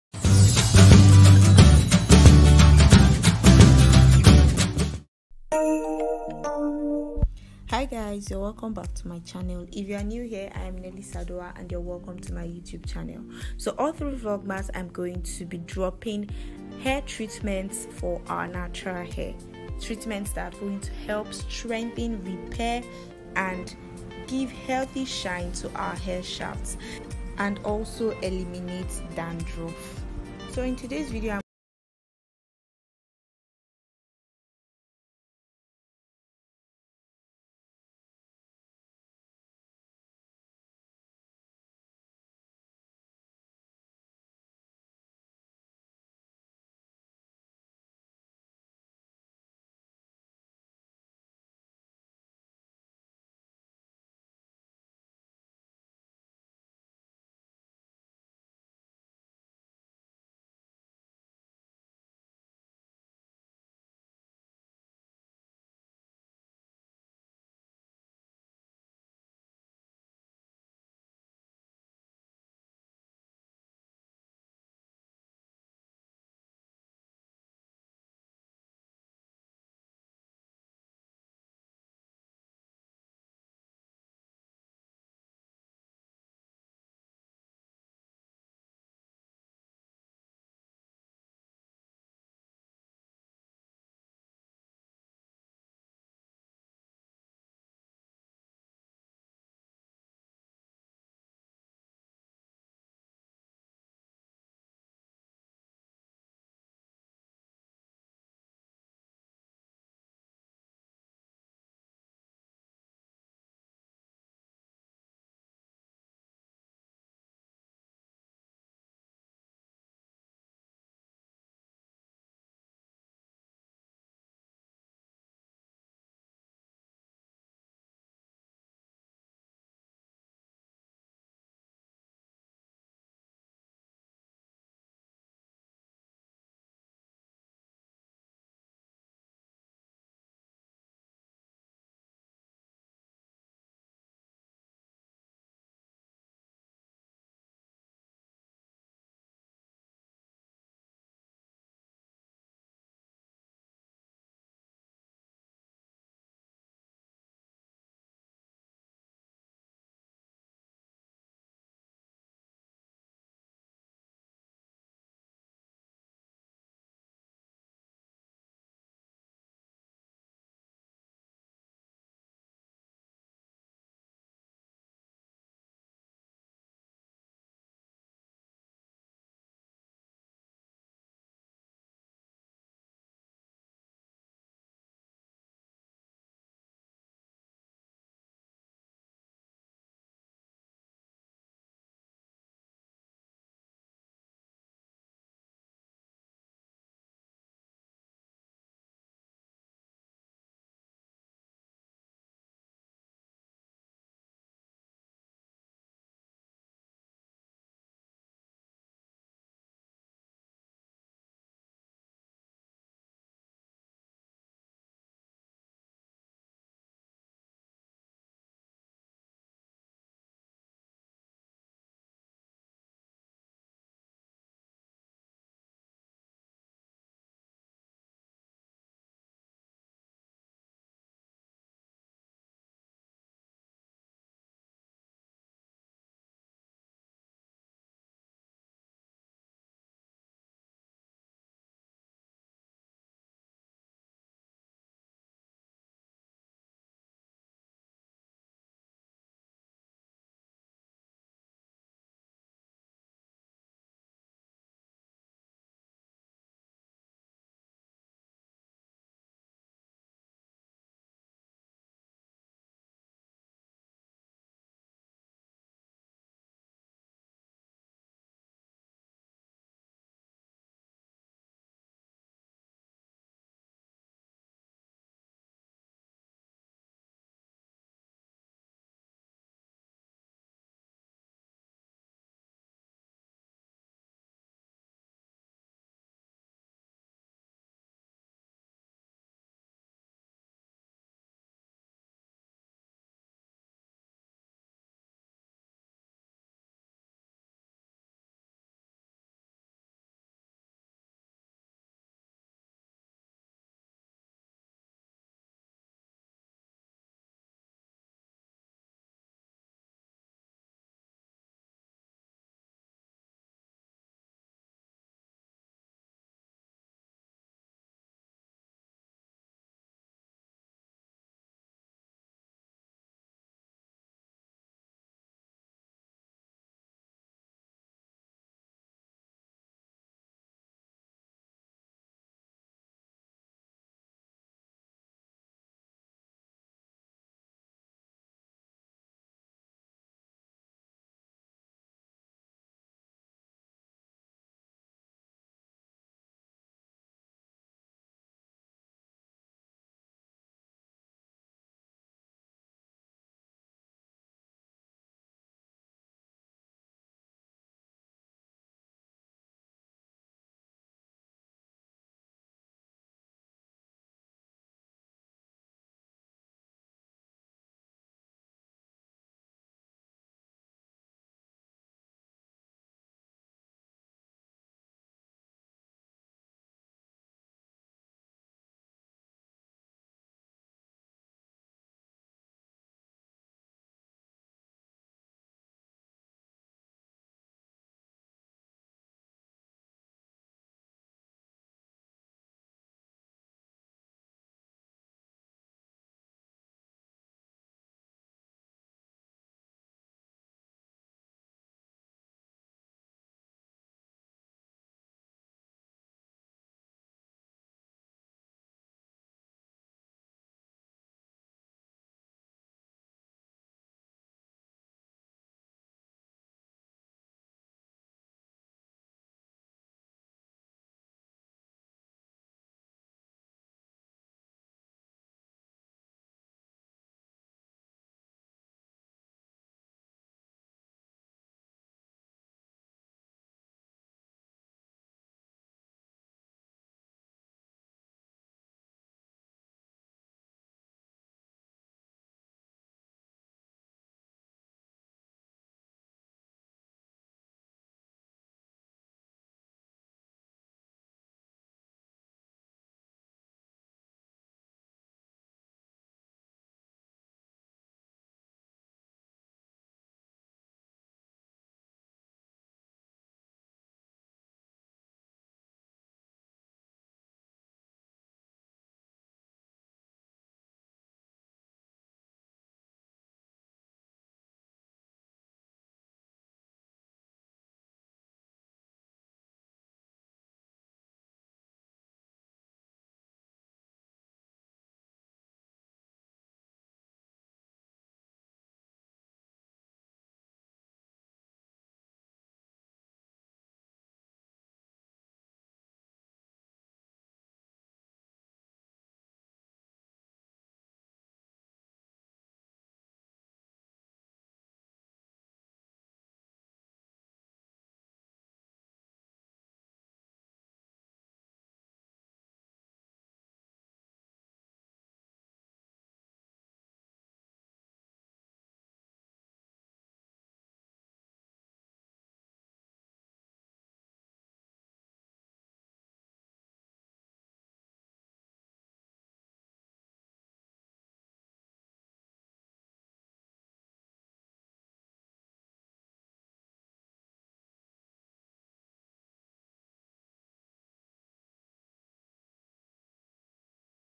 Cloves and neem hair spray treatment for natural hair
Hey loves
How are you all doing?
Trust you are doing great
In this vlogmas series your girl is back with another video .
In today's video I will be sharing this amazing hair treatment . The potent properties of these ingredients used for this hair spray will solve scalp issues such as dandruff,prevent hair loss,give healthy shine to hair strands,stop breakage and promote hair growth.

Ingredients
Cloves(zobo pepper)
Neem powder
Water

Hair treatment

Filming gears
Infinix hot 8
10 inches ring light
Editing App
Vita